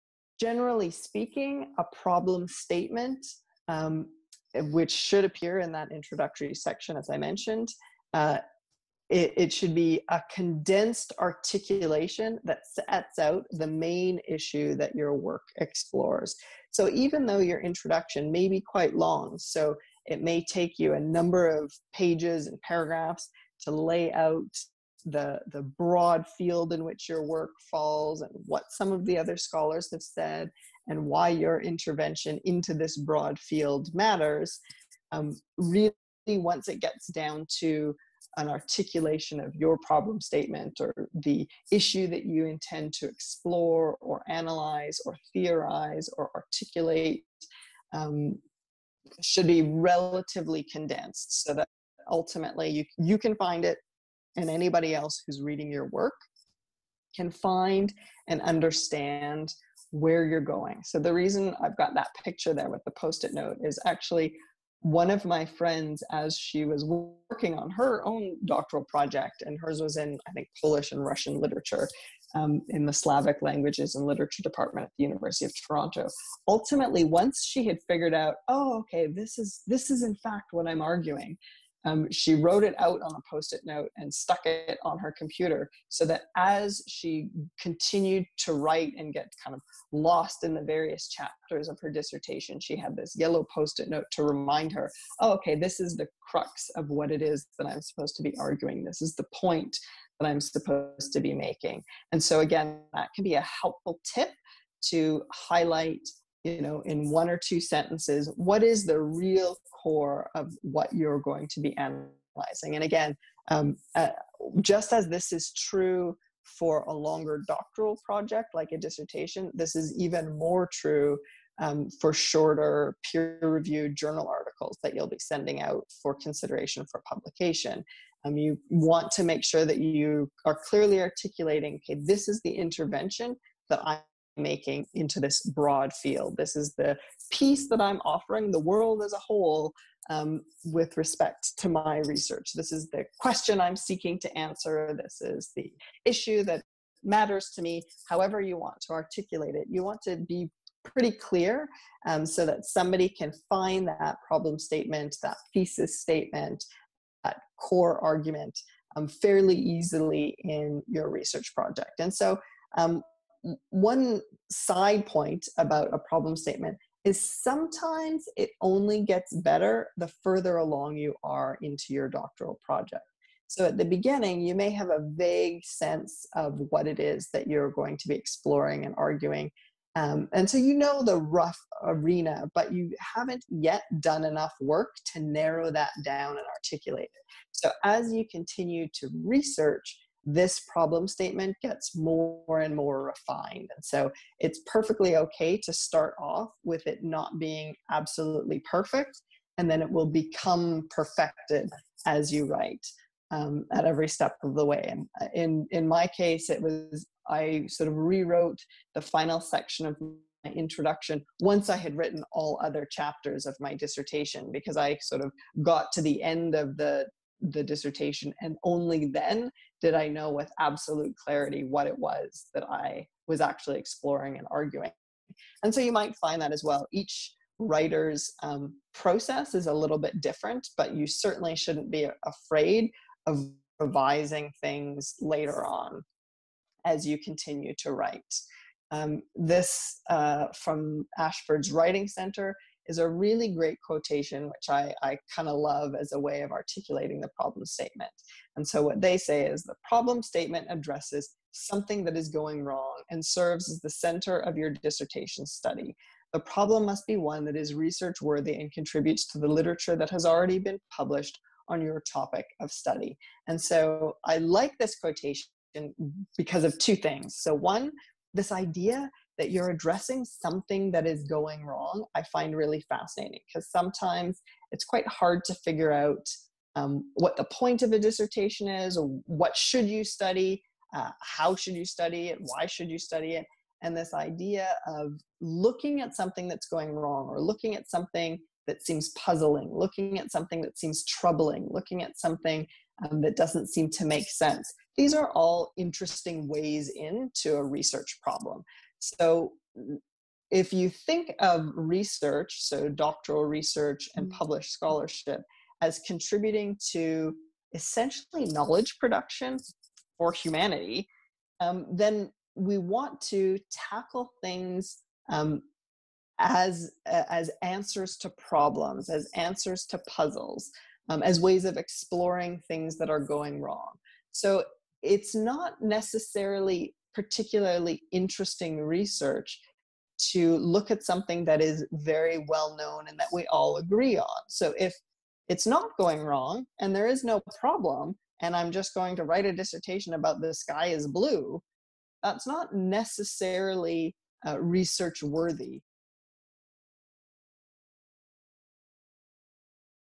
What is Your Problem Statement? | Thesis & Dissertation Writing
Problem statement or thesis statement? These are interchangeable and will form the core of your dissertation. It should immediately give a reader the how, what, and why of what your thesis addresses.